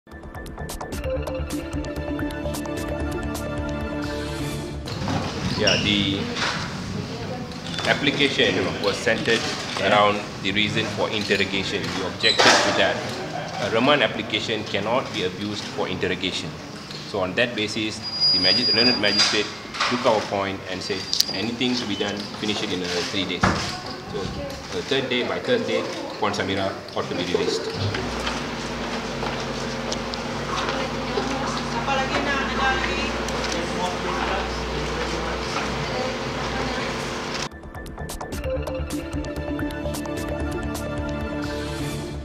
Lawyer to seek review of Cradle CEO’s stepsons’ remand
The lawyer acting for Cradle CEO Nazrin Hassan’s wife will file an application seeking a revision of the remand order issued for her two teenage sons.

LS Leonard said he is waiting for instructions from the family.

“The remand order can be revised, either to shorten it or revoke it all together,” he said after the Magistrate’s Court granted a three-day extension for the remand of Nazrin’s wife, Samirah Muzaffar.